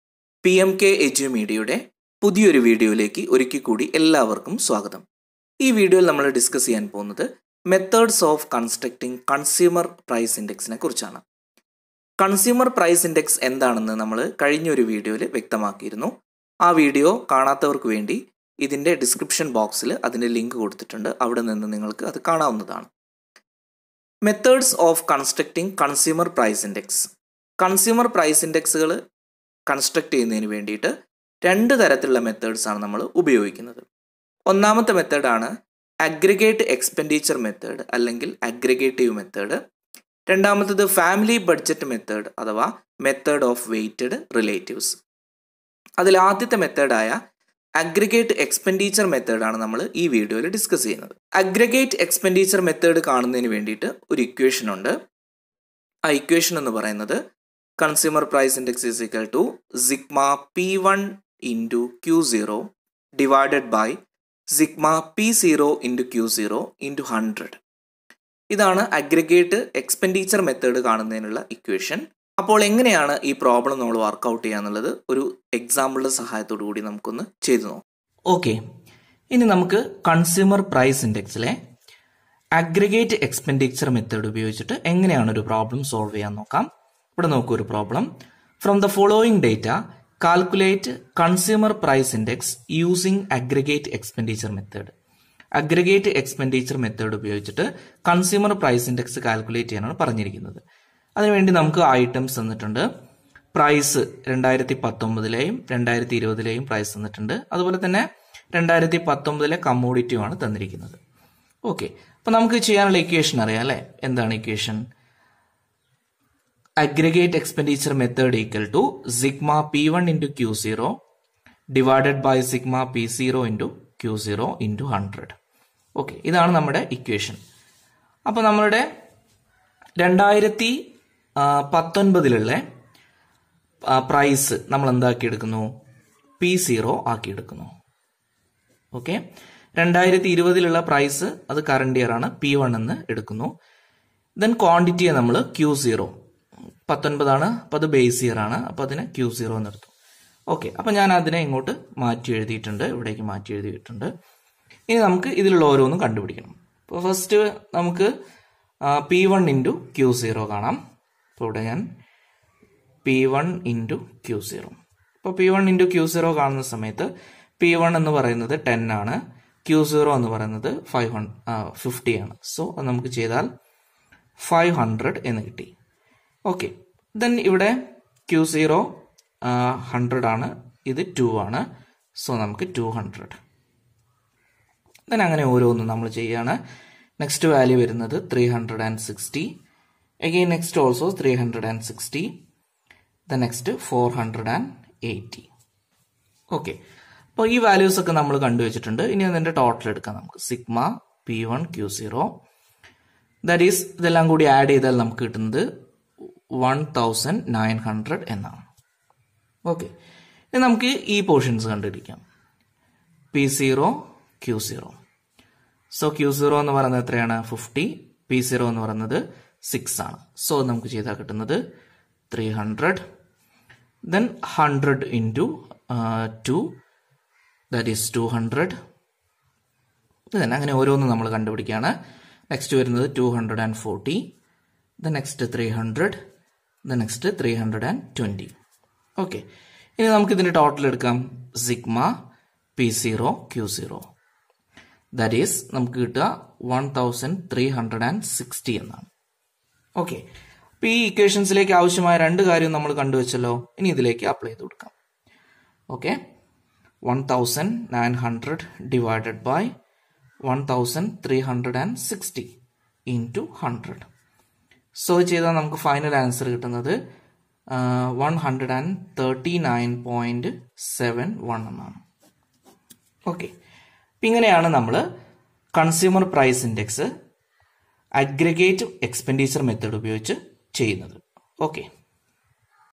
Aggregate Expenditure Method | Index Number
Content:
What is Aggregate Expenditure Method in Consumer Price Index Number .
Practical problem with solution.